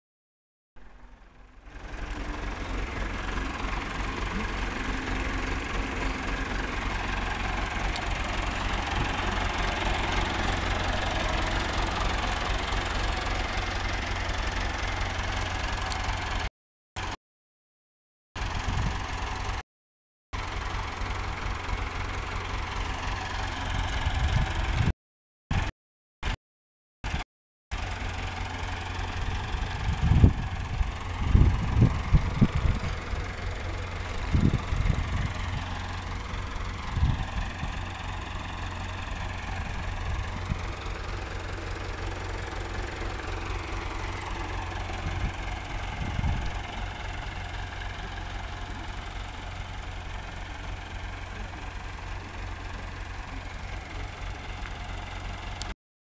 lindner 1650 mit landsberg pflug teil6 ende.
lindner traktor 1650 ist fertig mit den pflügen am acker,ca 1ha.ackerfläche abgeschlossen.